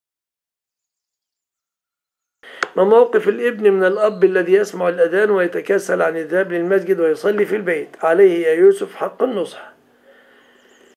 موقف الابن من الأب الذي لا يذهب للمسجد الشيخ مصطفى العدوي
ـــــــــــــــــــــــــــــــــــــــــــــــــــــــــــــــــــ
💎👐 مرحبا بكم في قناة DzEtude التعليمية  👐💎

                            🗼 أهم قوائم التشغيل 🗼

                           💎💡  ملاحظة   💡💎

 💖  لا تنسونا من دعائكم الصالح جزاكم الله كل خير  💖